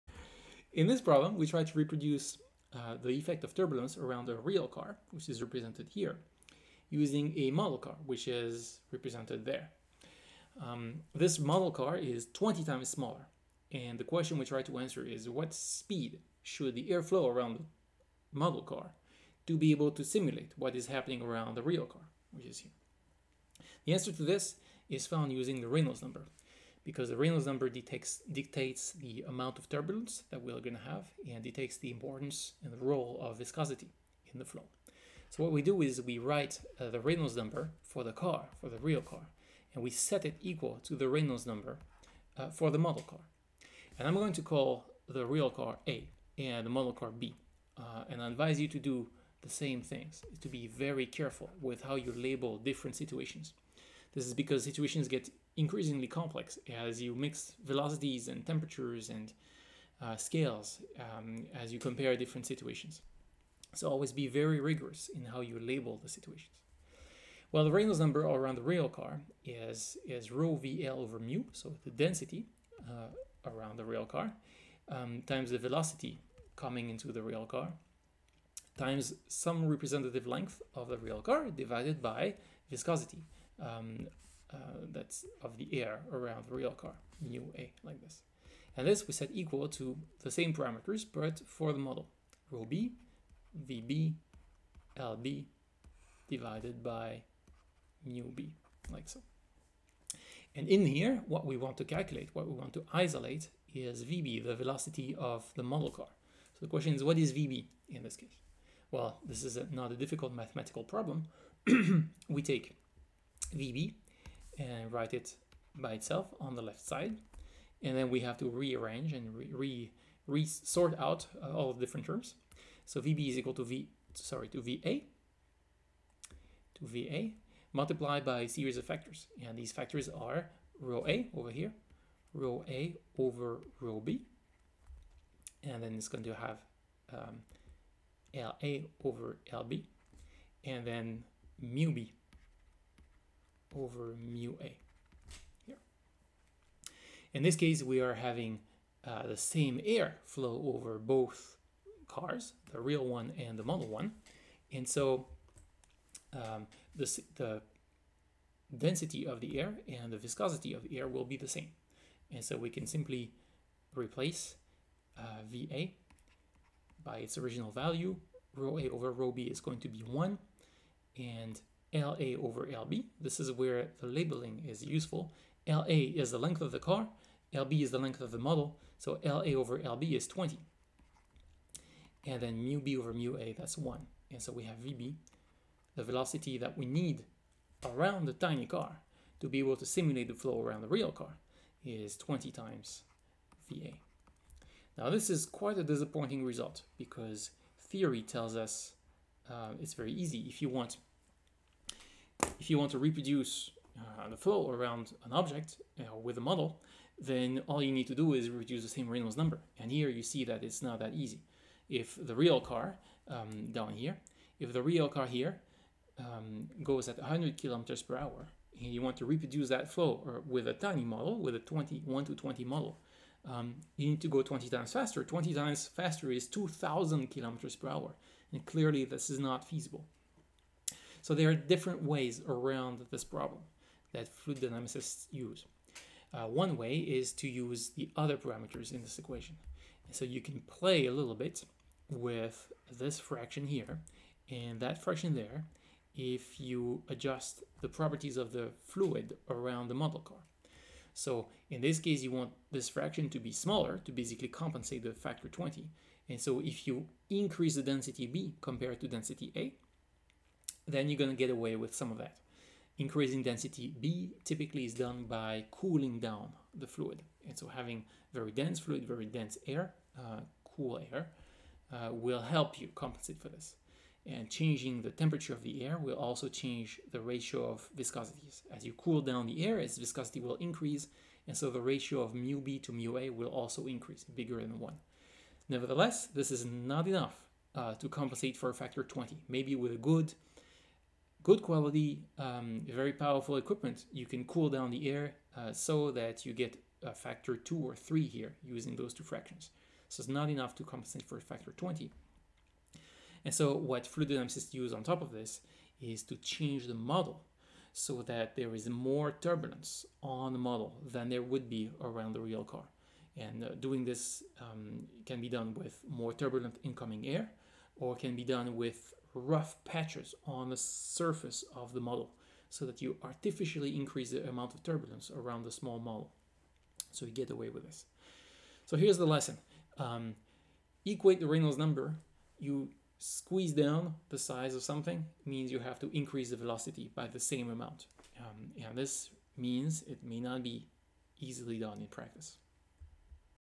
Scaling the flow around a model car (Fluid Dynamics with Olivier Cleynen)
How to determine the speed at which you need to run your wind tunnel experiment when using the model of a car.

Part of a series of theory and solved problem videos